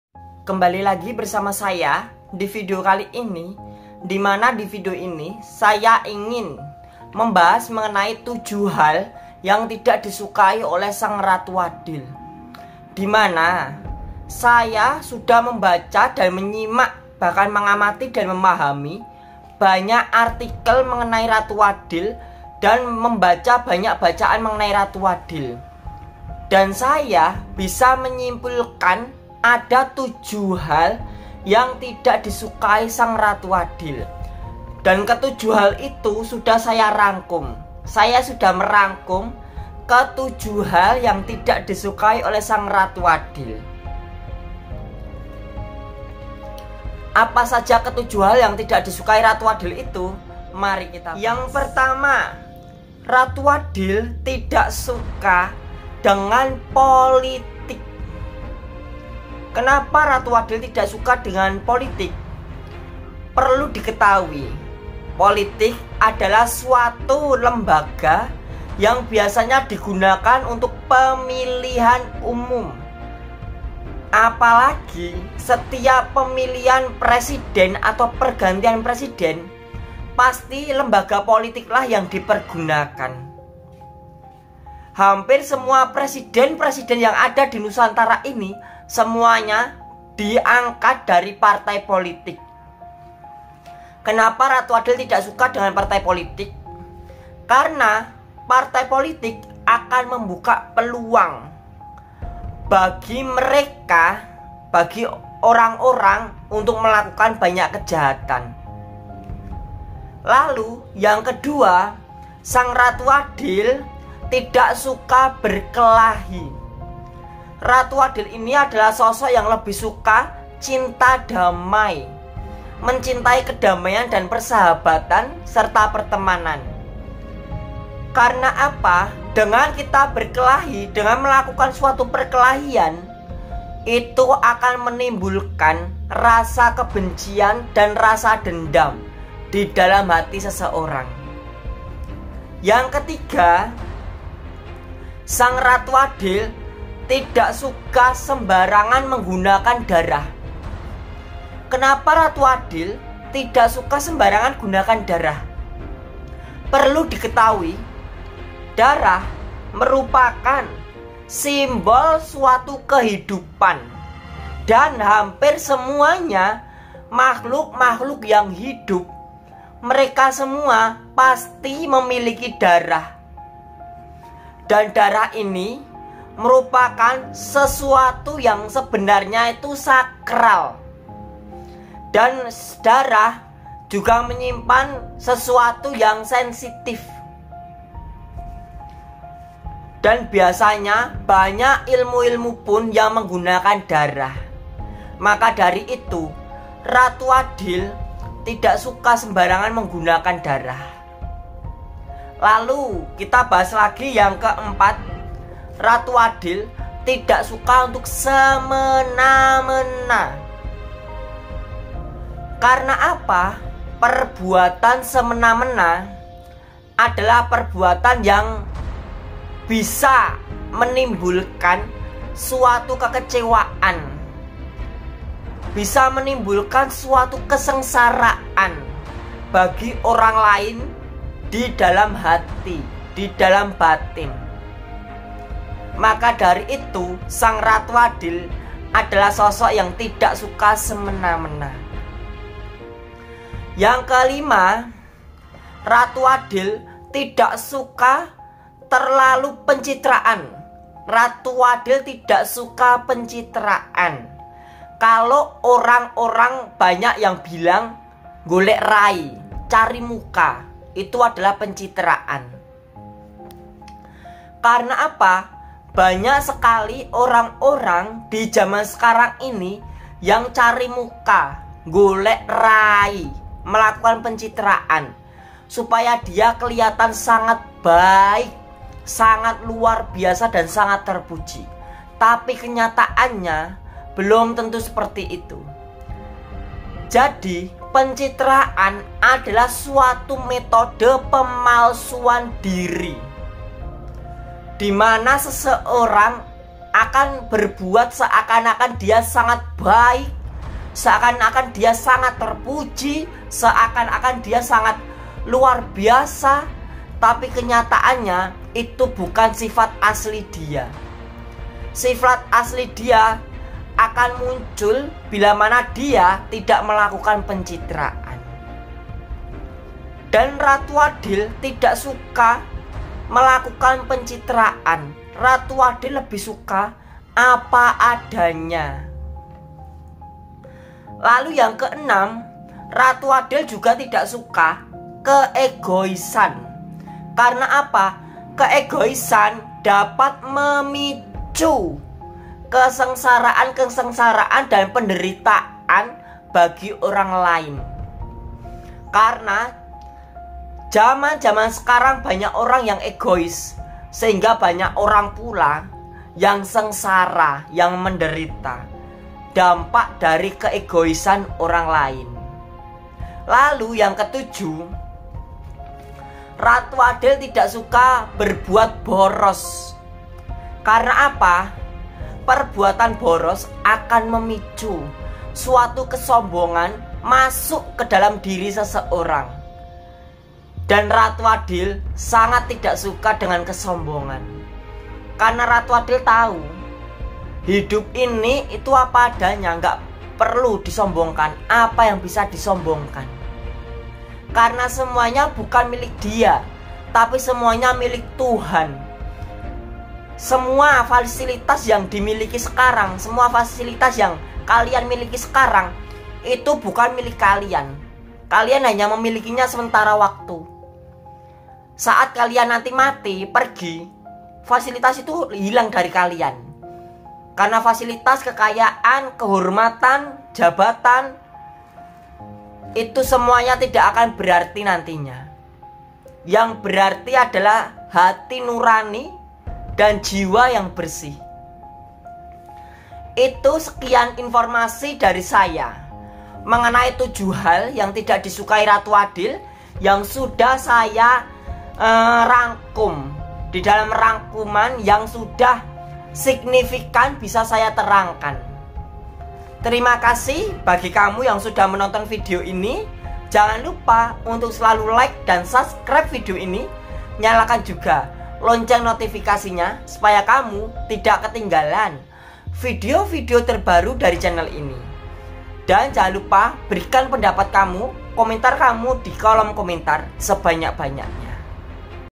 7 Hal Yang Dibenci Ratu Adil‼️Nggak Tau Rugiii‼️
7 Hal Yang Dibenci Ratu Adil‼️Nggak Tau Rugiii‼️

Vidio ini menjelaskan tentang tujuh hal yang dibenci oleh ratu adil atau satrio piningit.